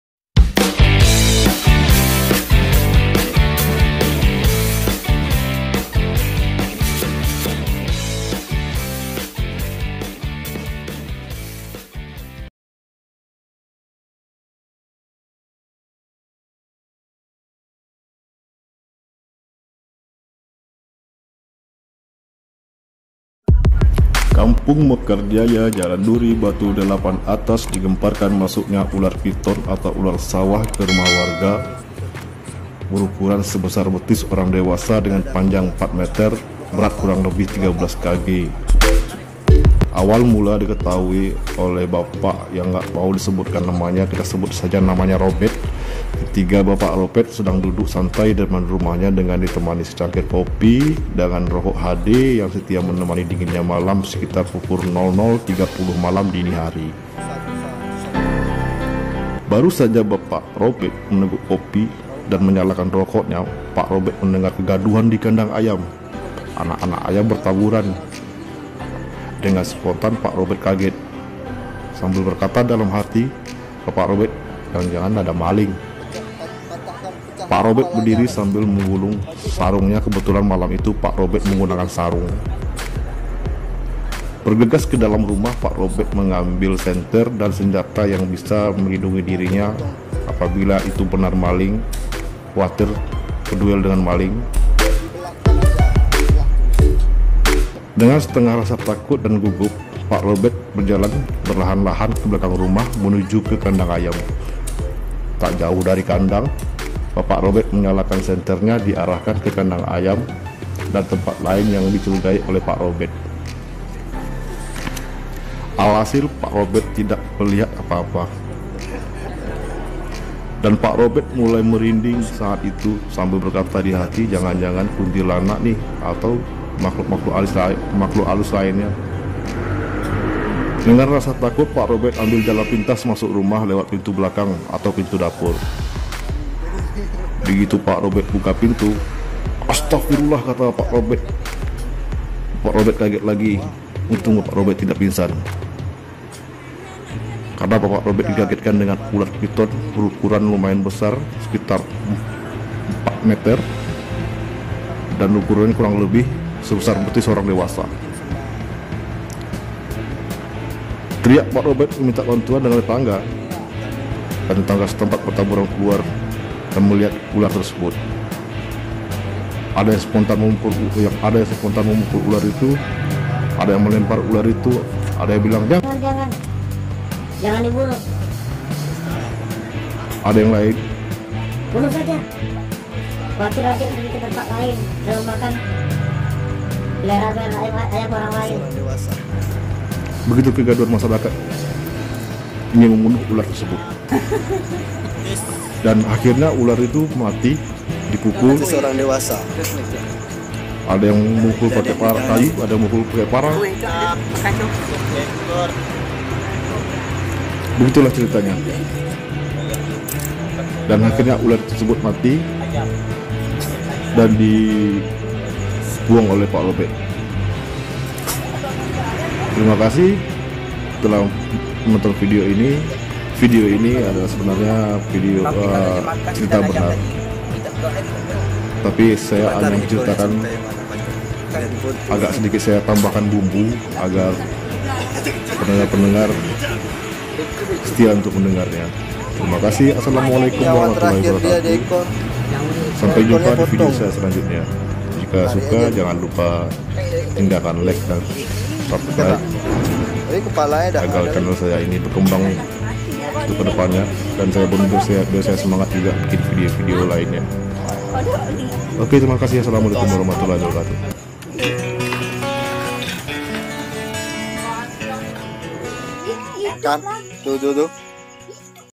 ULAR PITON BERUKURAN 4 METER MASUK RUMAH WARGA ~ Batu 8 atas tanjungpinang
ini adalah kisah benar,kejadiannya tgl 15 Mei 2021,di batu 8 atas jl.nuri tanjungpinang ular berukuran 4m masuk kerumah warga.saya tambahkan sedikit bumbu untuk di bacakan agar sesikit menarik untuk di dengar.